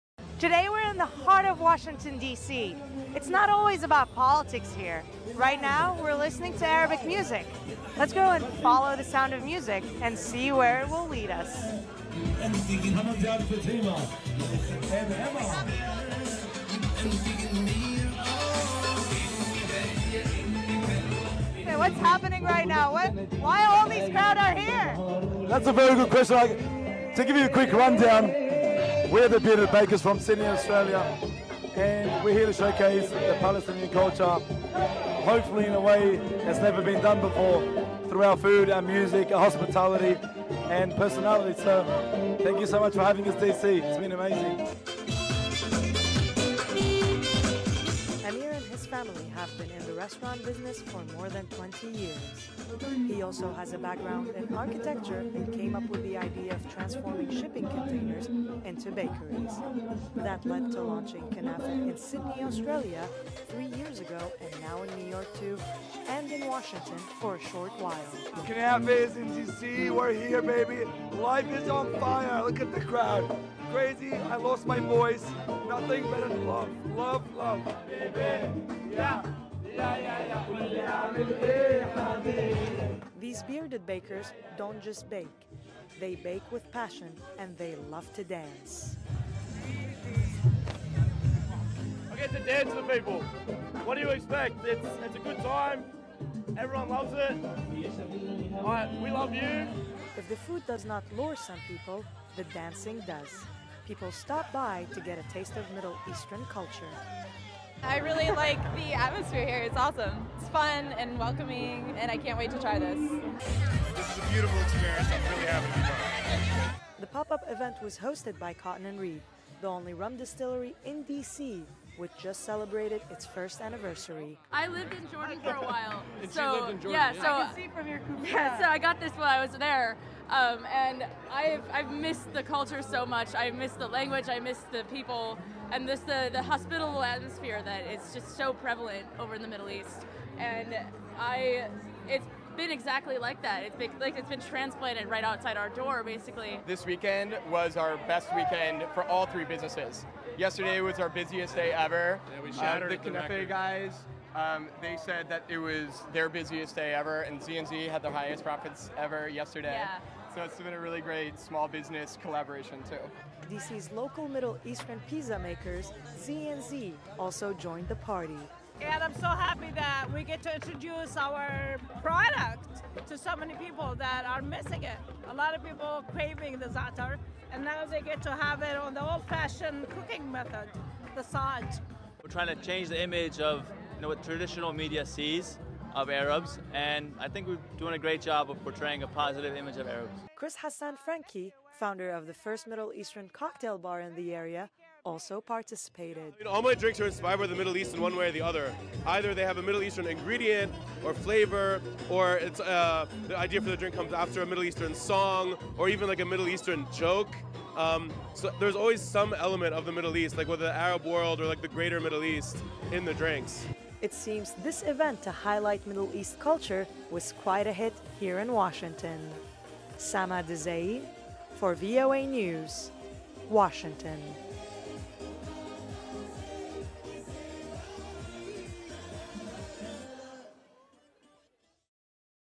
Middle Eastern Food and Block Party a Hit in Washington
You’ll hear the music they play before you even see them. Coming all the way from Australia, this group of young Middle Easterners wants to showcase Middle Eastern hospitality through their dessert. Sama Dizayee reports.VOA NEWS